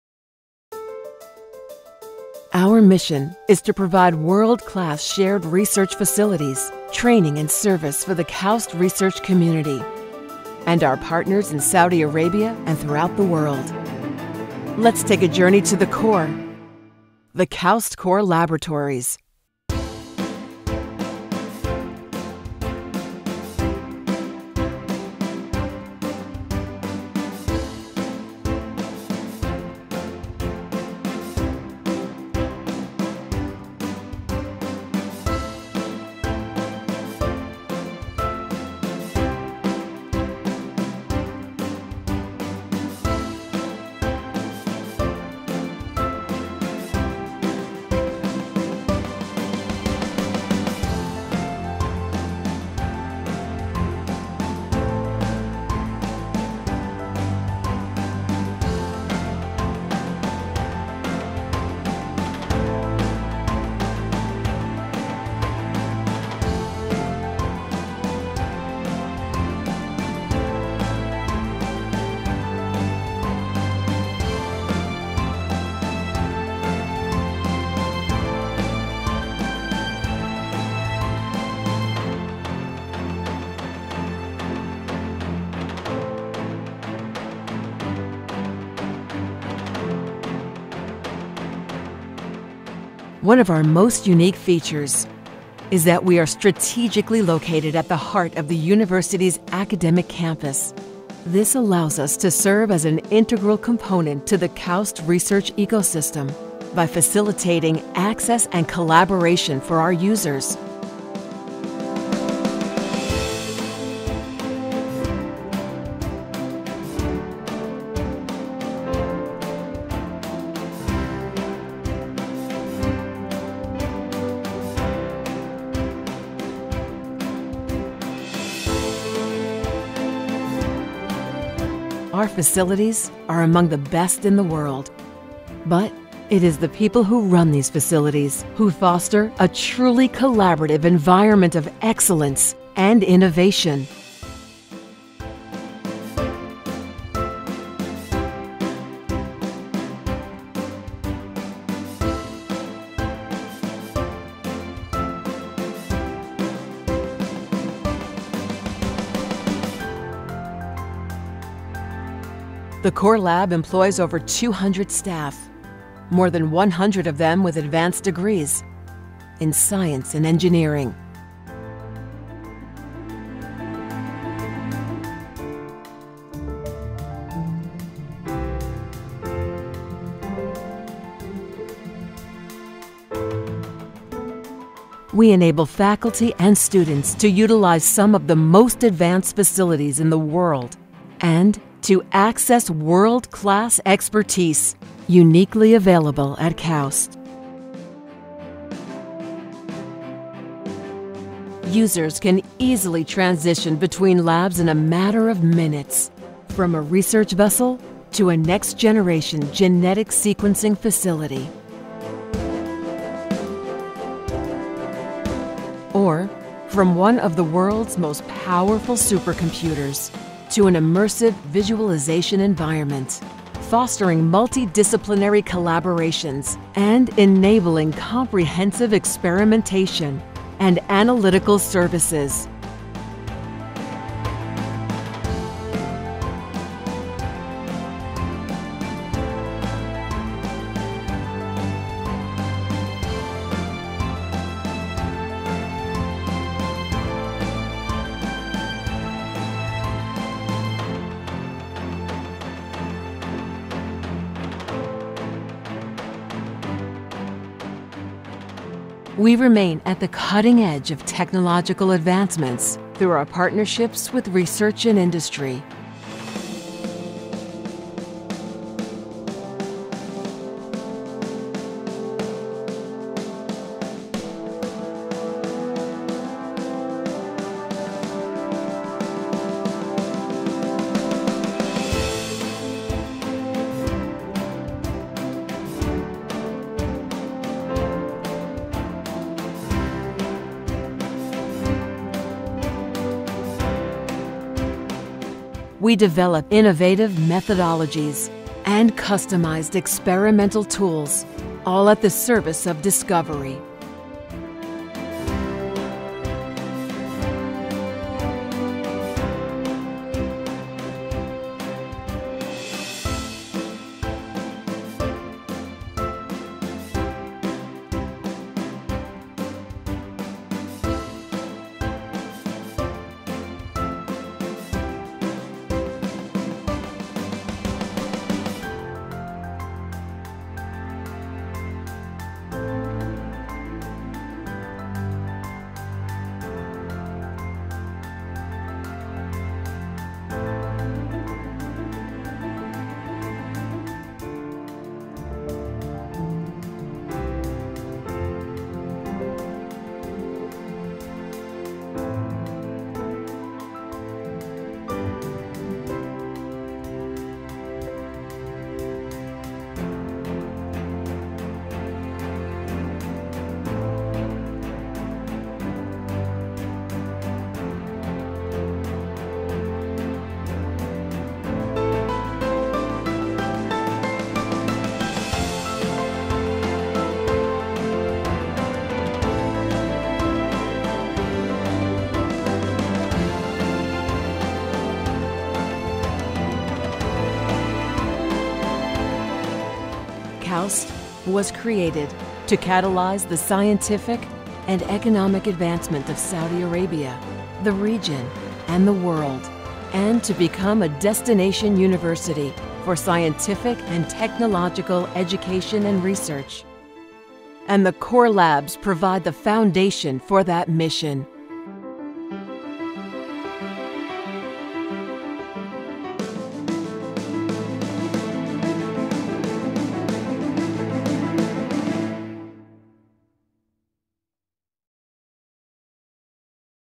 KAUST Core Labs and Research Infrastructure: EXCELLENCE FUELED BY EXPERTISE AND TECHNOLOGY
KAUST’s major research facility, Core Labs and Research Infrastructure is a system of multidisciplinary, interconnected laboratories located on the University's campus. Its mandate is to promote the scientific ambitions of KAUST and the Kingdom of Saudi Arabia by stewarding and developing state-of-the-art facilities and technical expertise. Outstanding Core Labs scientists, engineers, and technicians provide research support, training, collaboration, and services, enabling discovery for the Kingdom and the world.